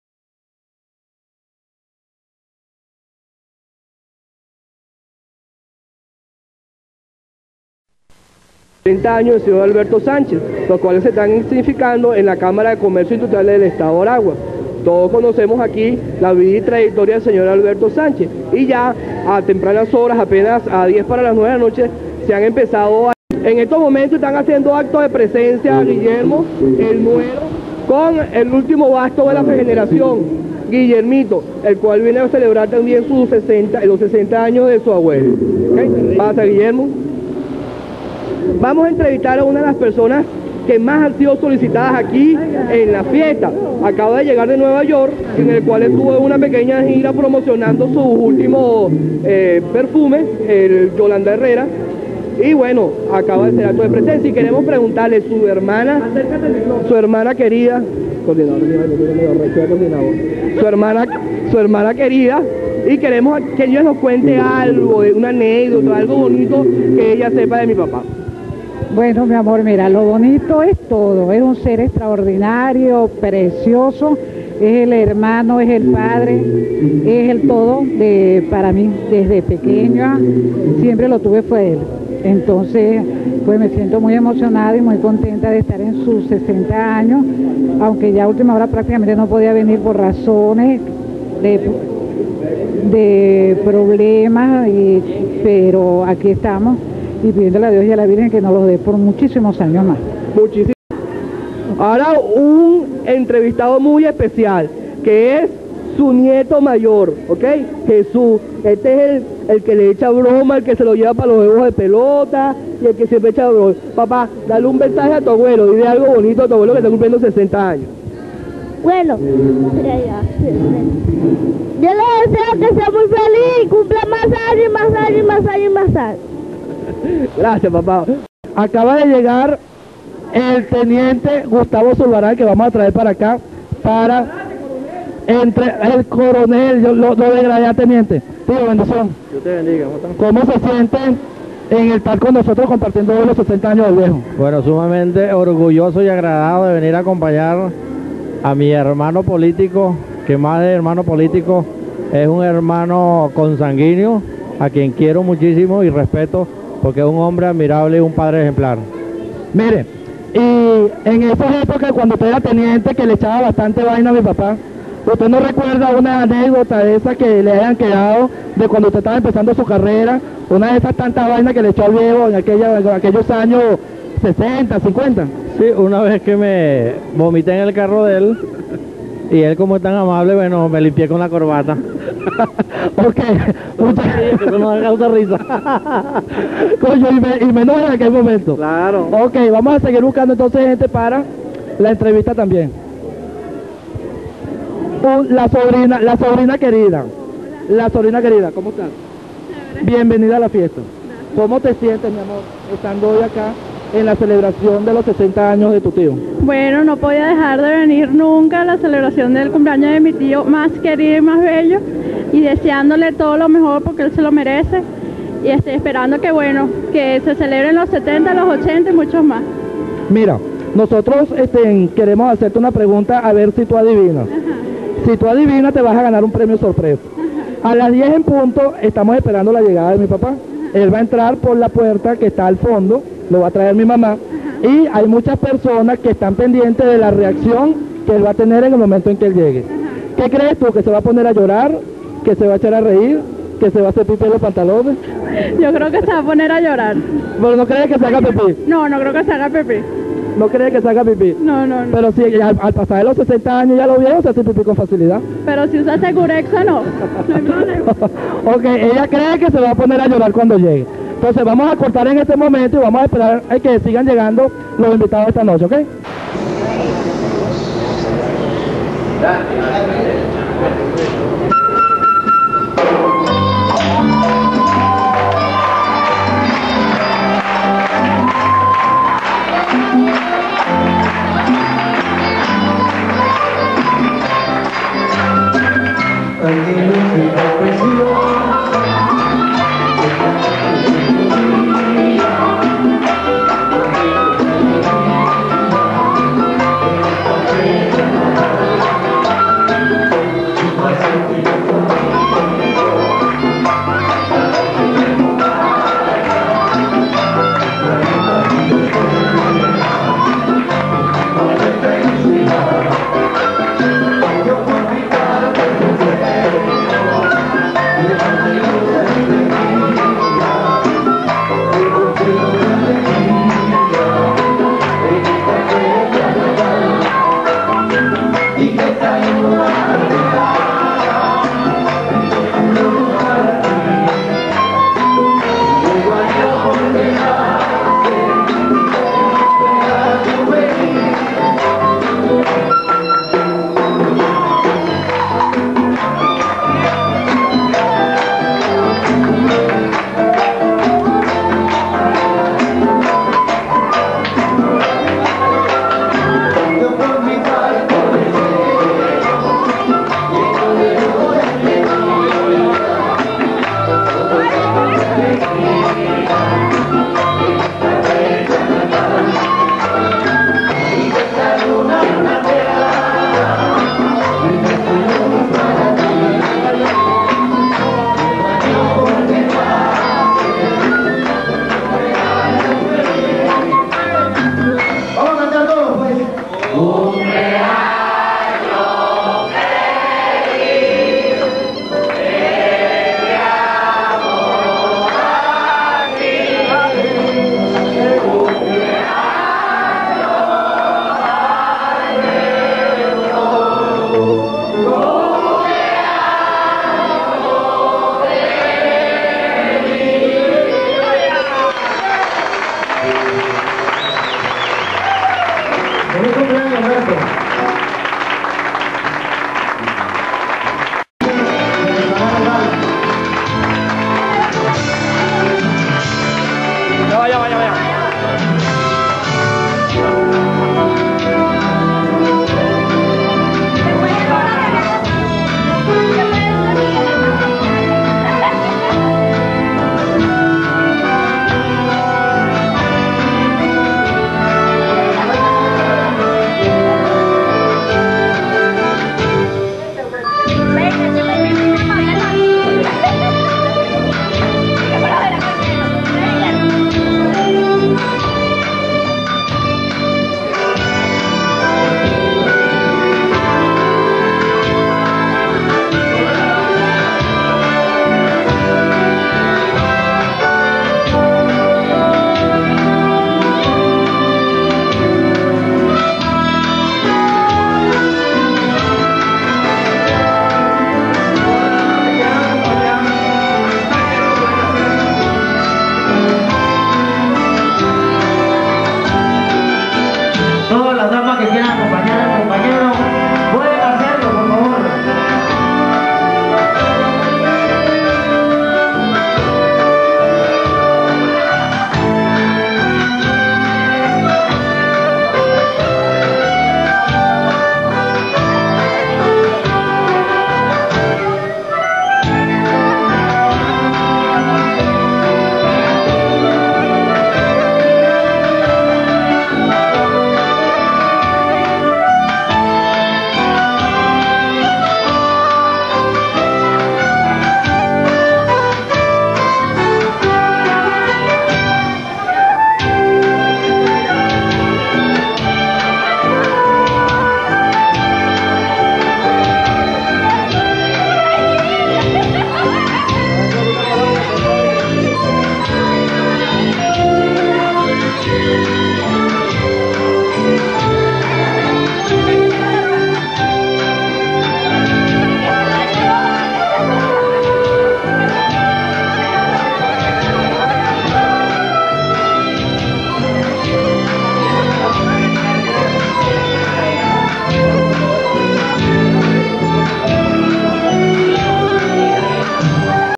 Familia Sánchez Sulbarán
Celebración 60 años Alberto Sánchez en la Cámara de Comercio del Estado Aragua, Maracay, Venezuela